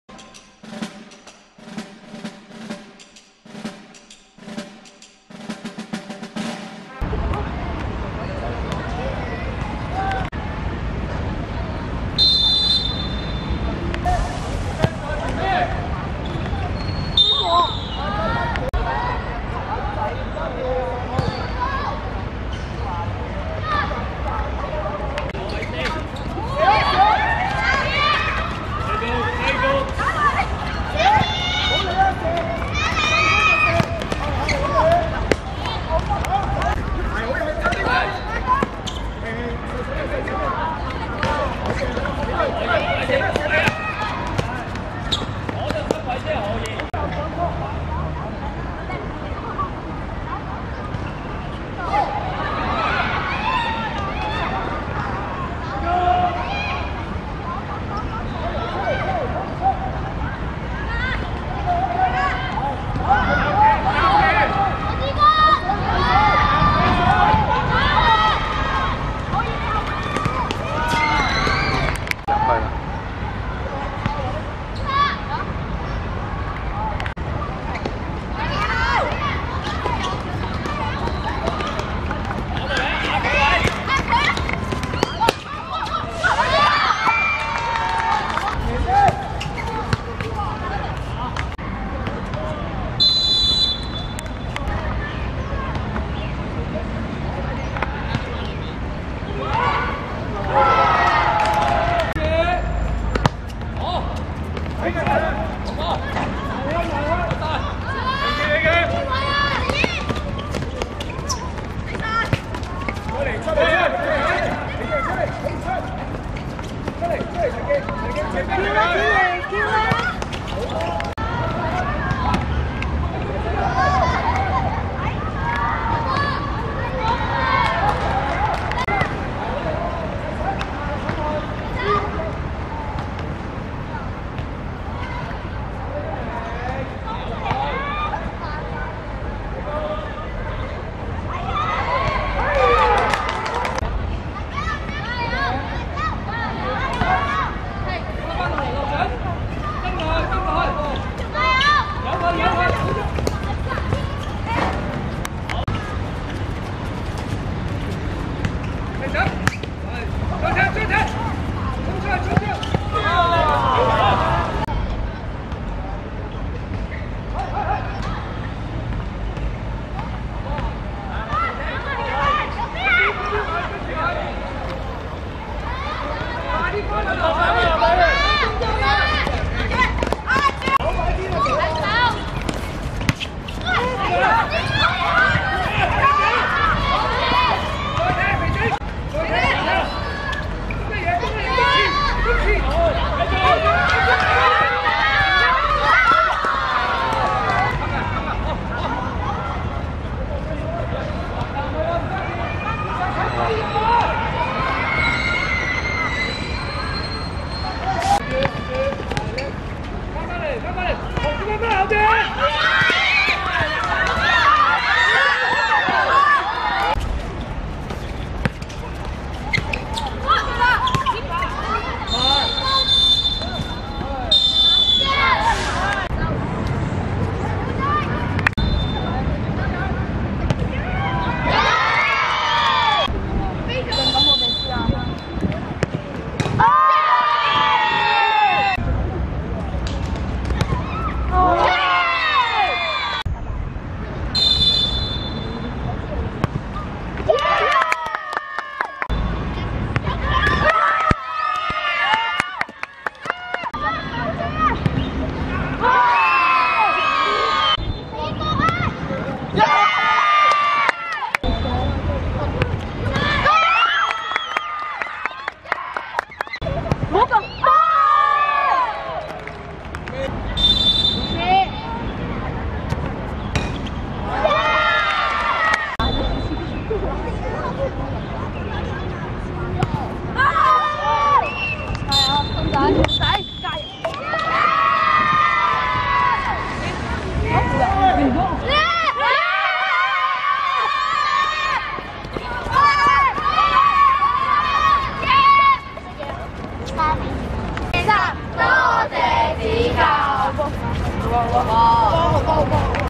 惠州vs馬錦燦(2016.12.13.北區小學足球決賽)精華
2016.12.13.北區公園足球塲 11:00
上水惠州公立學校(紅)~(3) 0 : 0 (2)~東華三院馬錦燦紀念小學(藍)